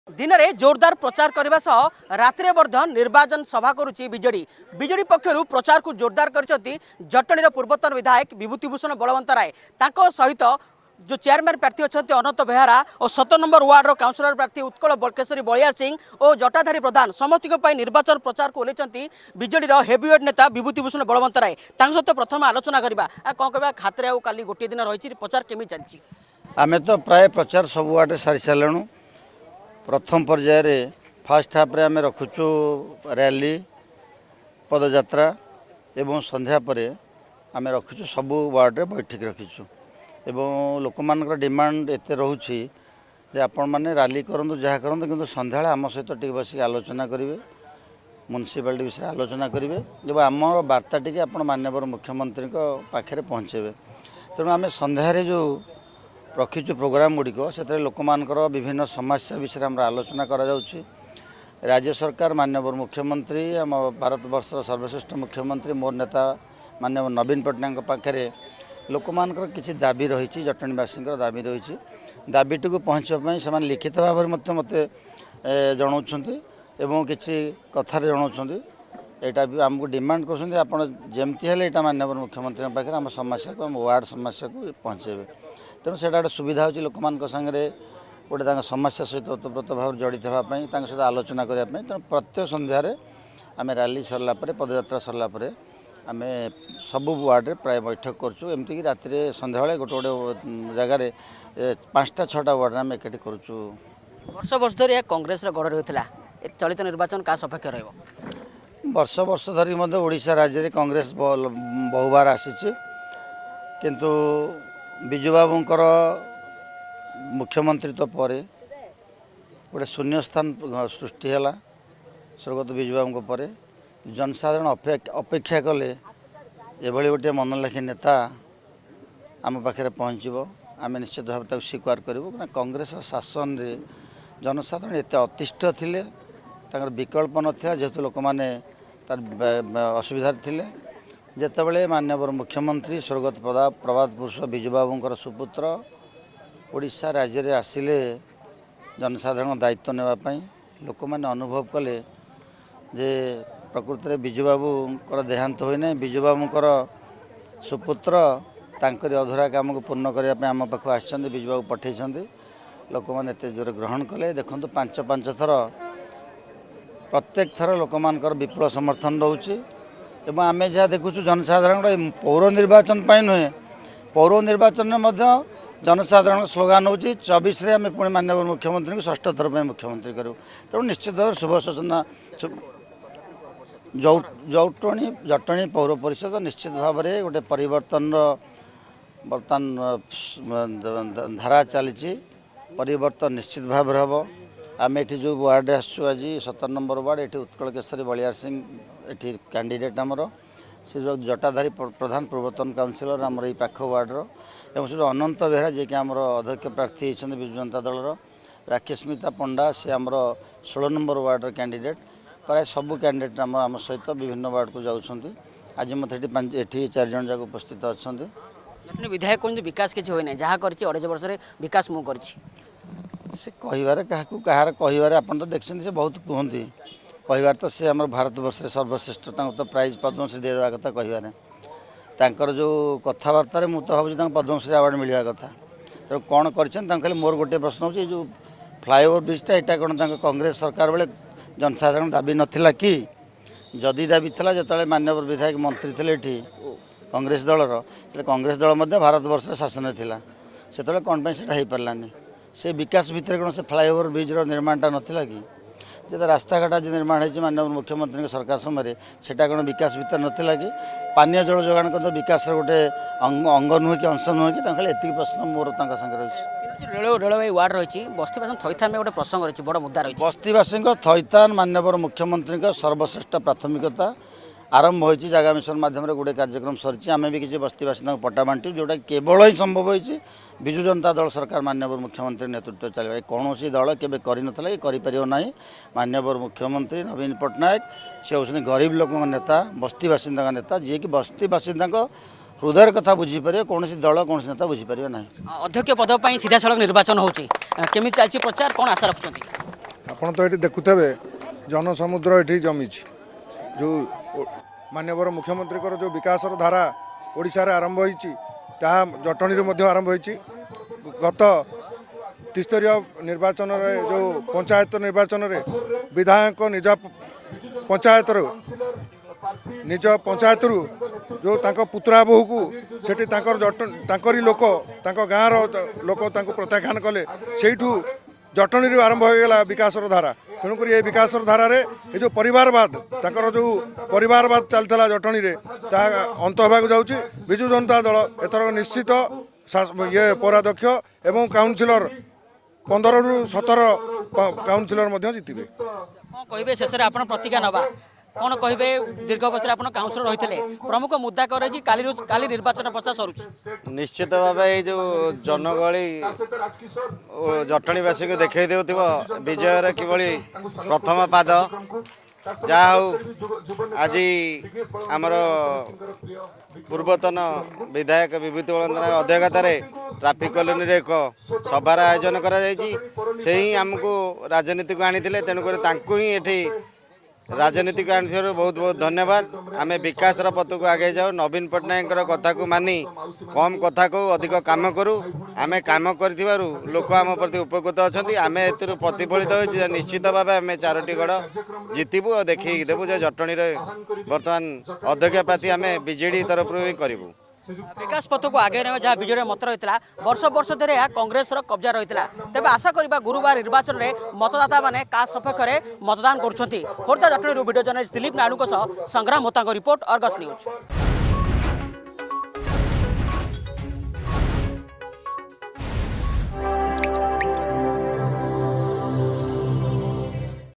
Odisha Urban Polls 2022: Former BJD MLA of Jatani hits Campaign trail for Party candidate in Khordha
The election campaign is in full swing by Jatani's former BJD MLA Bibhuti Balbantray for the party candidates in Khordha ahead of municipal elections 2022.